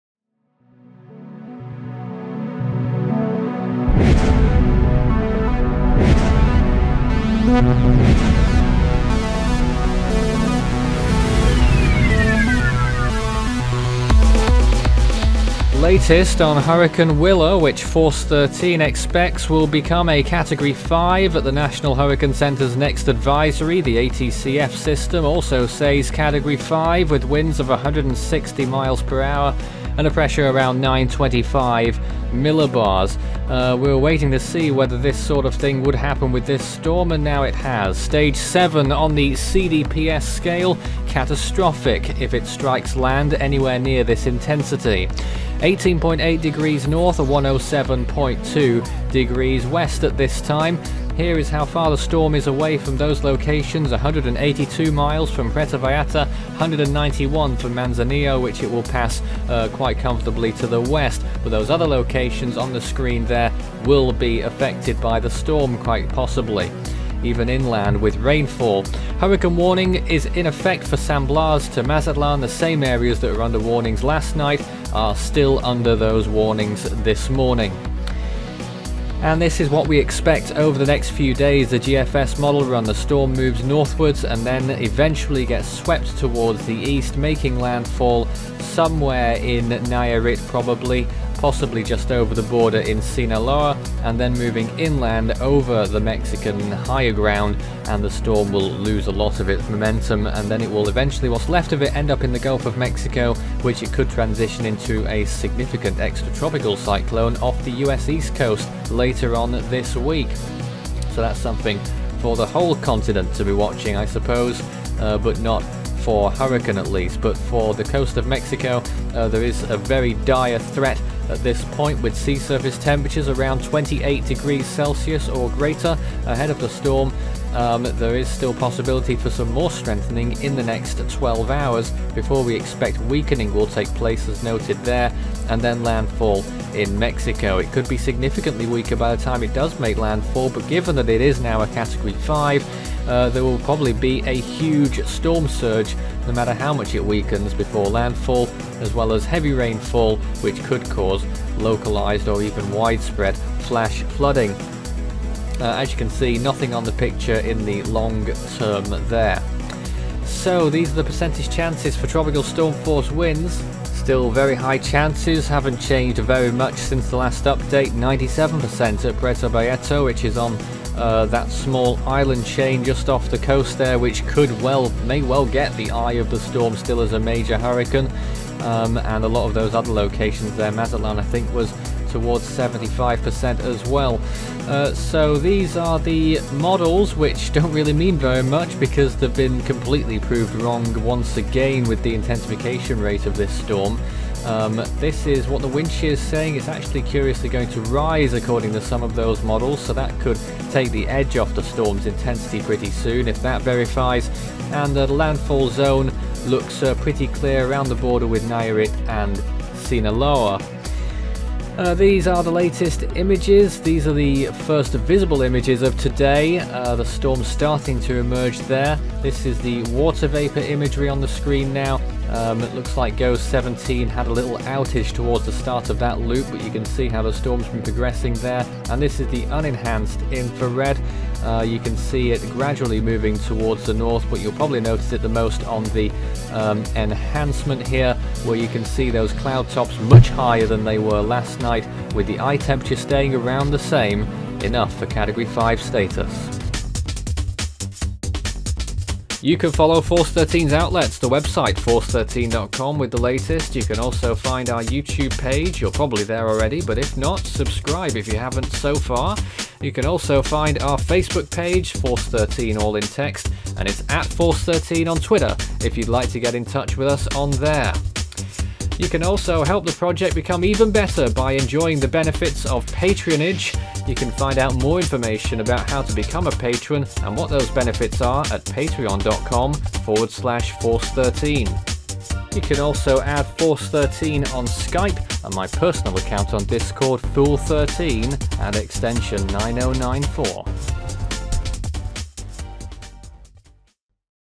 Hurricane Willa still strengthening - 8am MDT Oct 22, 2018
Force Thirteen's latest estimate, along with the ATCF system, has put Willa at Category 5 status with winds of 160mph and a pressure of 925mb. The NHC is likely to follow suit at the next update shortly or later in the day. Regardless of intensity, Willa is a very powerful storm and has the potential to cause catastrophic damage to the coast of Jalisco, Nayarit, and Sinaloa with its winds, storm surge, and heavy flooding rains.